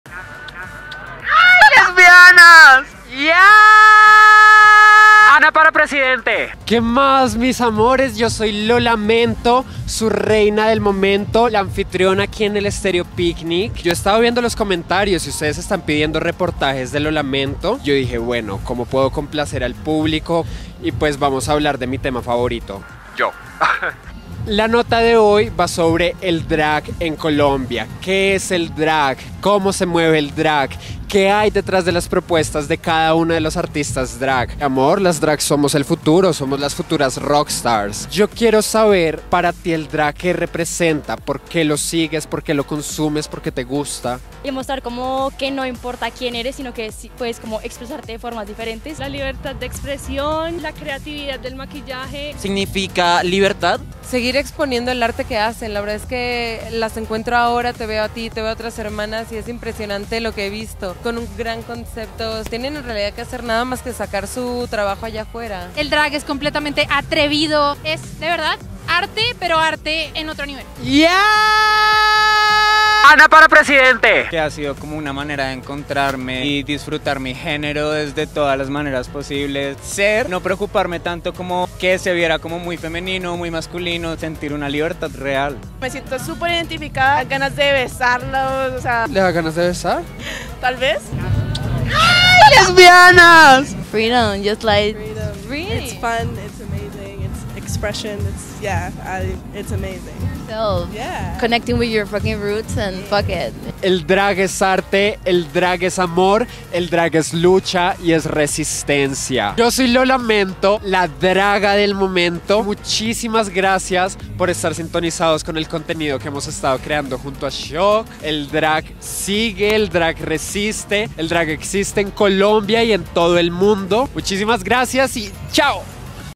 Lola Mento en Estéreo Picnic 2023: el drag en Colombia - Shock
El drag ha sido para muchos refugio, expresión artística, y entretenimiento. ¿Qué signifca para los asistentes de Estéreo Picnic 2023? Esa fue la pregutna de Lola Mento, la drag del momento en el festival.